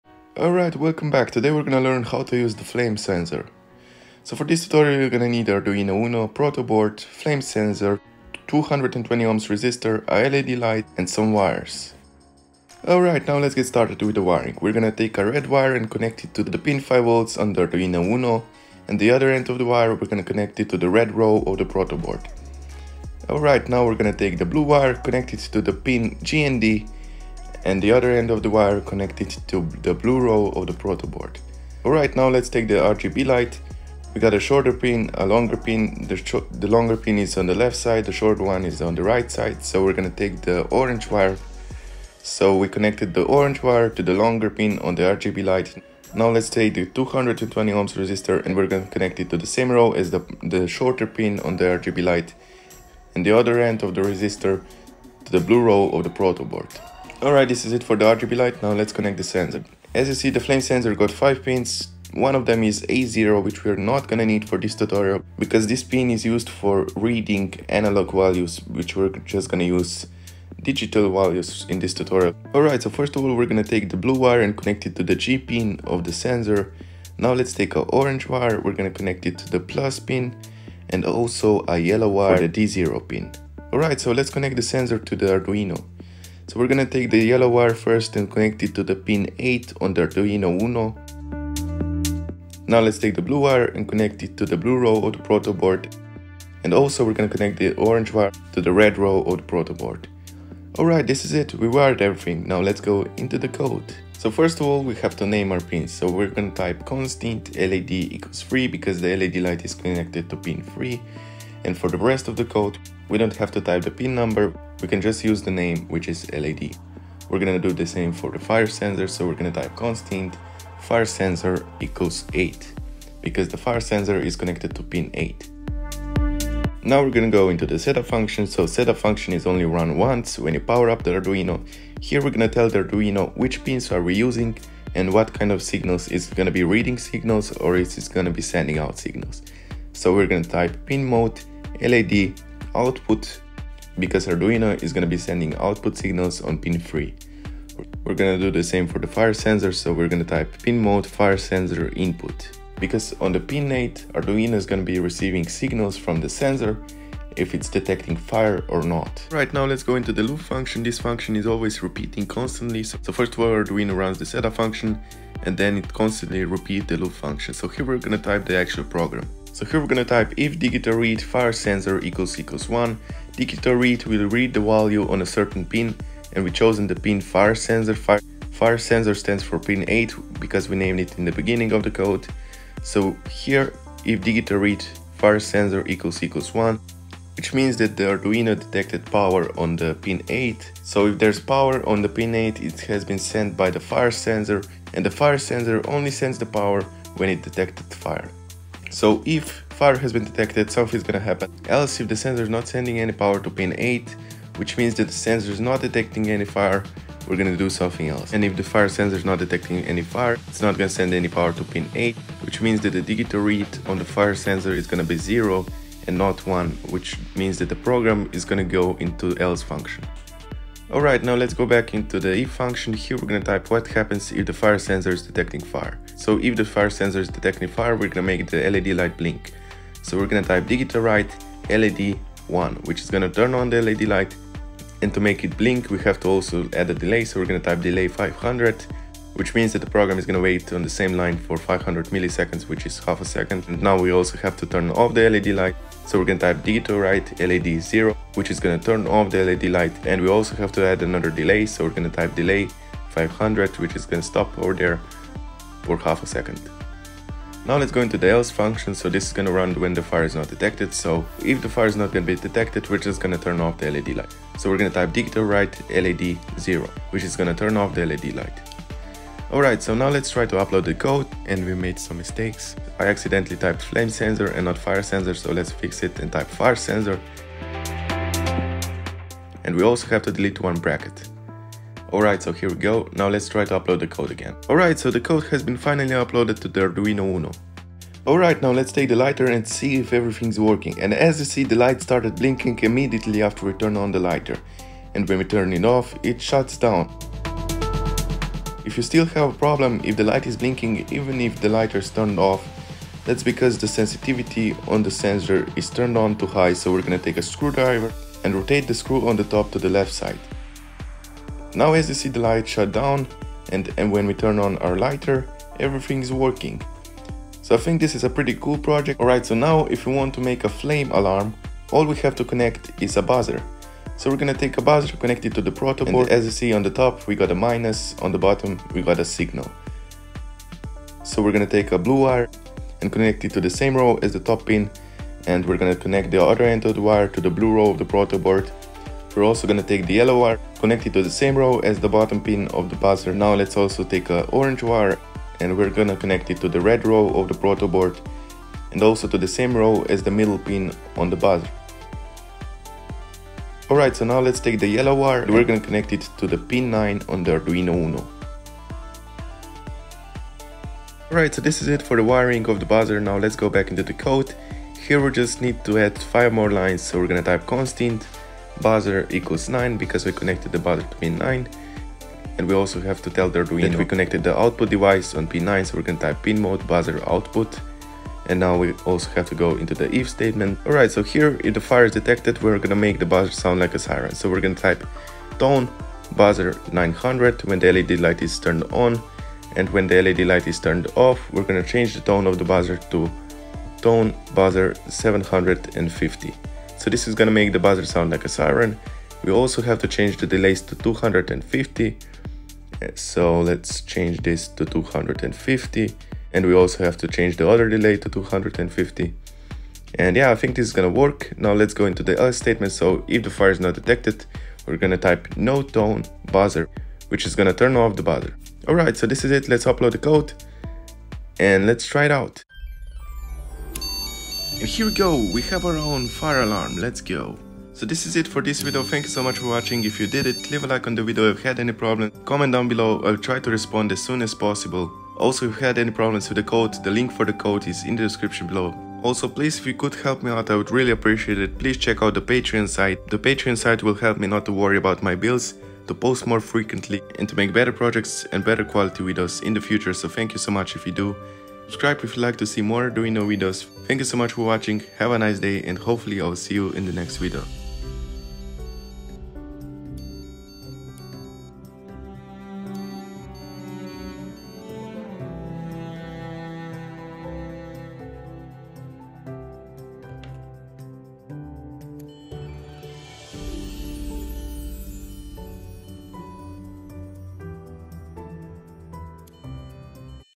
Build a Fire Alarm with Arduino: Step-by-Step Guide for Beginners
In this video, we'll show you how to build a fire alarm system using an Arduino board, a flame sensor, and a buzzer. We'll start with the basics, including how to wire up the components and write the code. Then, we'll put it all together to create a working fire alarm that you can use to protect your home or workspace. This tutorial is perfect for Arduino beginners, so grab your starter kit and let's get started!